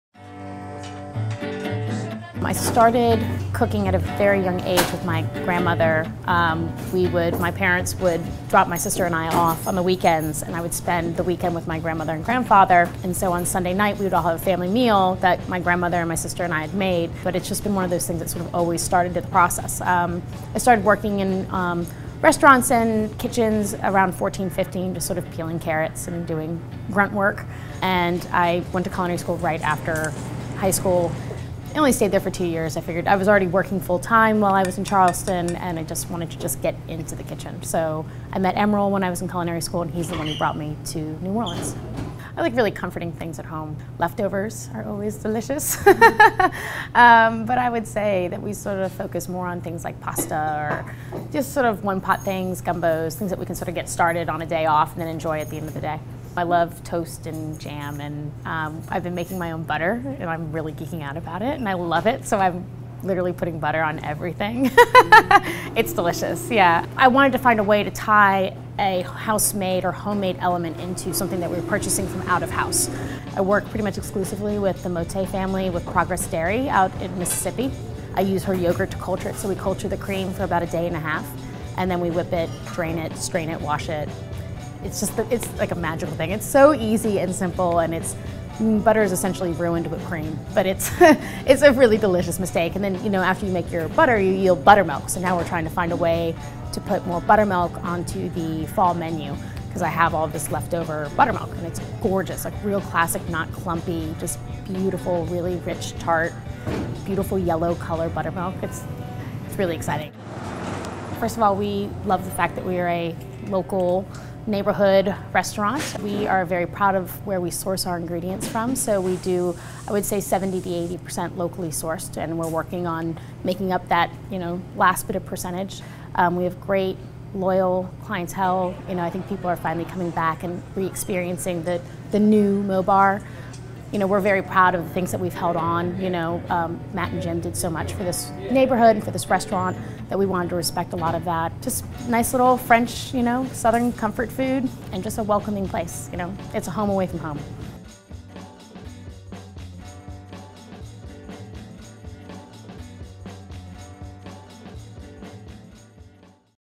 Meauxbar chef Kristen Essig
Meauxbar chef Kristen Essig talks about her grandmother's influence in her cooking path and what she's been trying out at the recently re-opened restaurant.